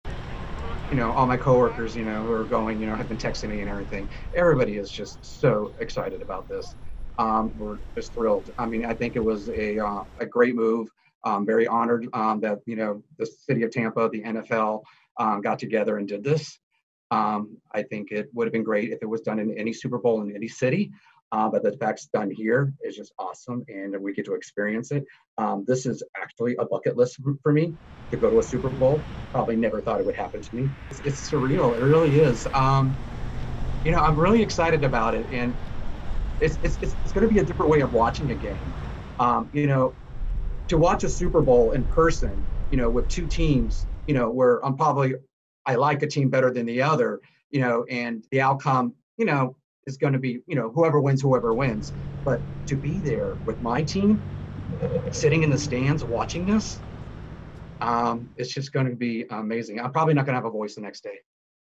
NFL gives Super Bowl tickets to hospital workers
The NFL says 7,500 health care workers vaccinated for the coronavirus will be given free tickets to this year's Super Bowl in Tampa, Florida. (Feb. 5)